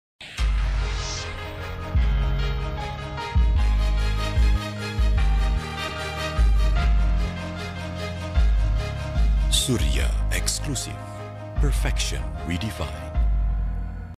Iklan Surya Exclusive - Perfection Redefined (2014) @ Trans TV, Indosiar, SCTV, RCTI, & ANTV
Iklan ini pernah ditayangkan di Trans TV, Indosiar, SCTV, RCTI, & ANTV pada tahun 2014.